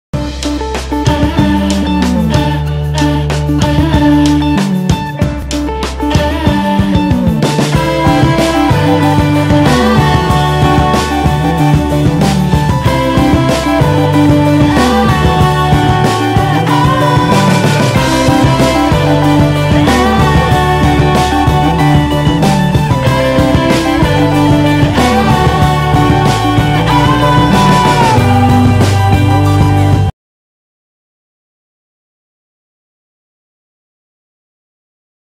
CRISPY PORCHETTA CROSTONE by Chef -Salvatore Cuomo-
The Sunday Brunch at -Salvatore Cuomo- restaurant